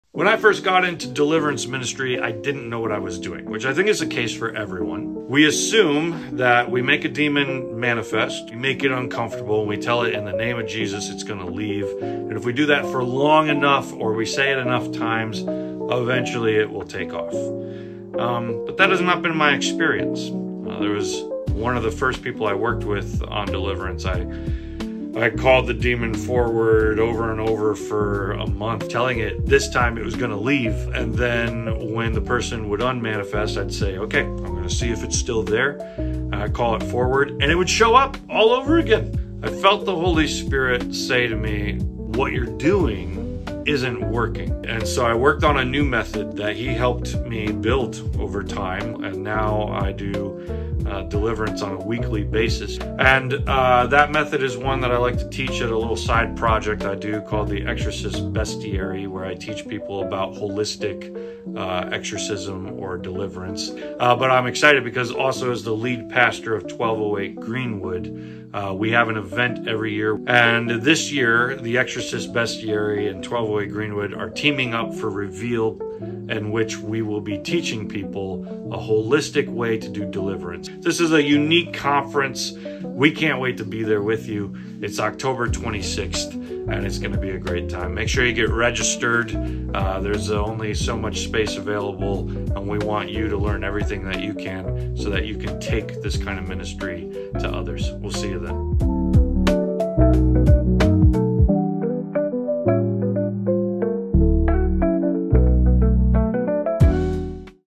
Reveal Conference on Deliverance Ministry!
Reveal Conference is right around the corner! Join us on October 26th as Pastor Jamin teaches us his holistic model of deliverance, which is a ministry he now participates in every week. Make sure to register soon, as space is limited